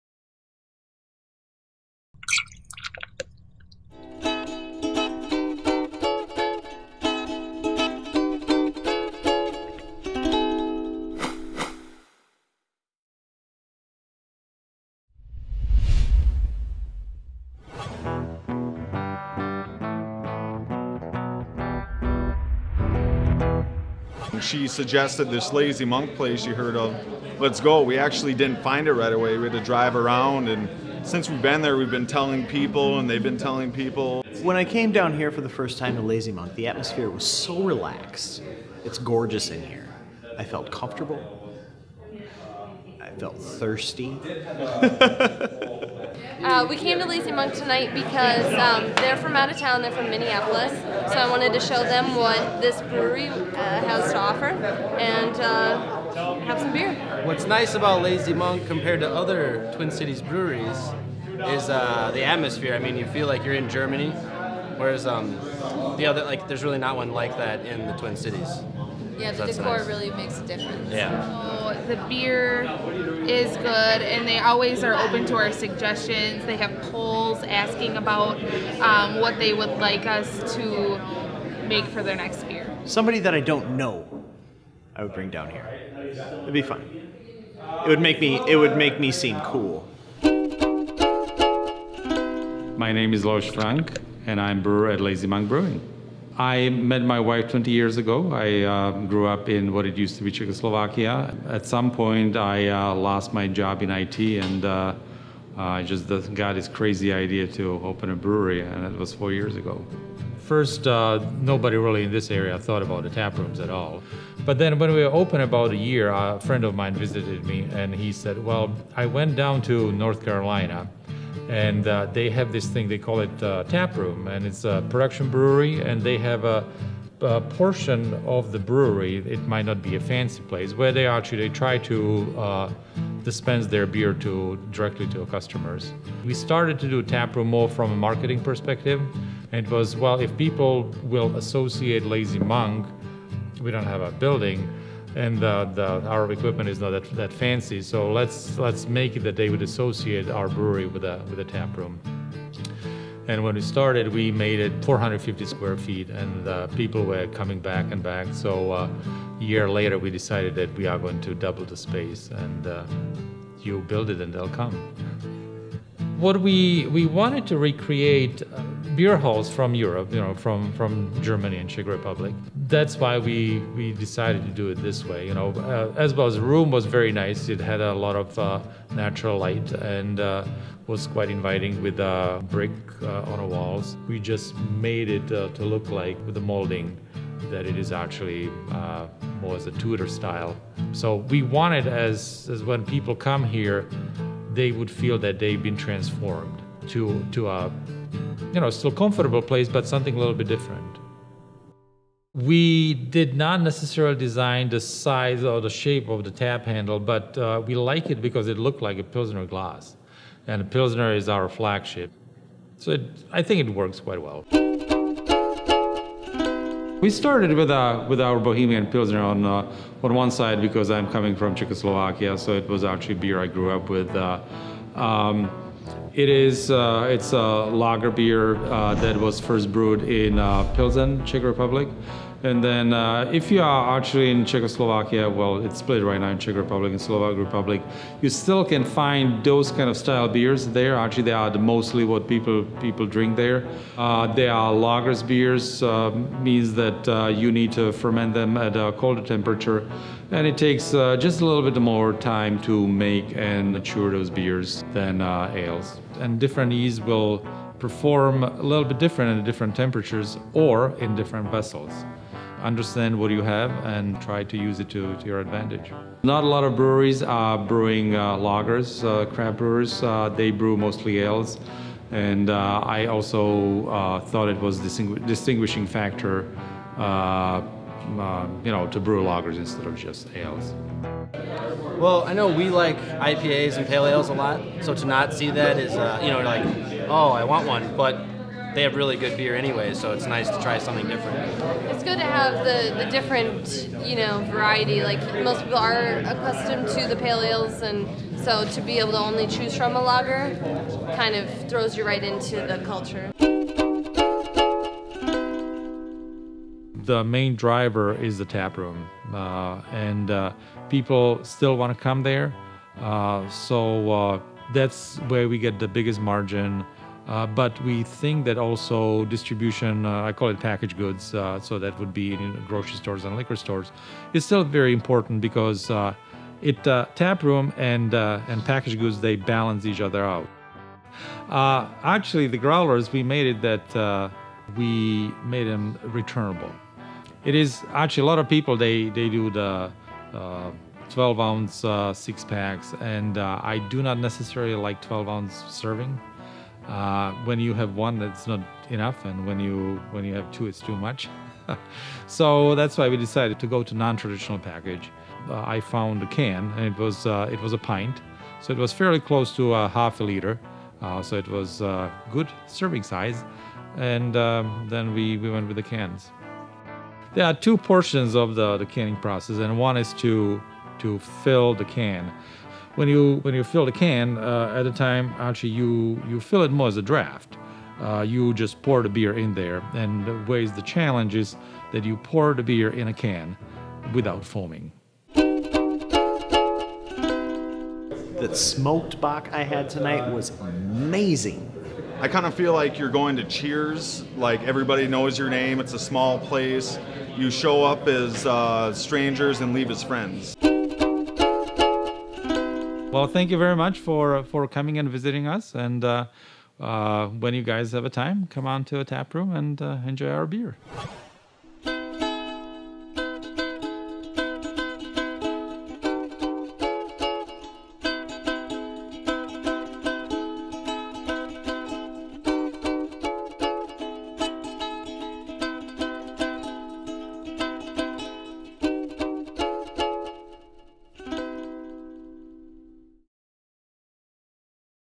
Taproom Travelers - Craft Beer Show: Lazy Monk Brewing
Craft Beer in Eau Claire, Wisconsin. In Episode 1 of Taproom Travelers we visit Lazy Monk Brewing. Lazy Monk is a Craft Brewery built out of a desire to serve the midwest with quality biers made in the old world tradition of the Czech Republic and Germany. Although they specialize in Bohemian and German beer styles they do not shy away from ales meeting the needs of beer drinkers with a variety of palettes. Come visit their European style taproom and find out what happens when community and friends meet over amazing biers brewed in the old world tradition!

-Taproom Travelers: Tapping the midwest one craft beer at a time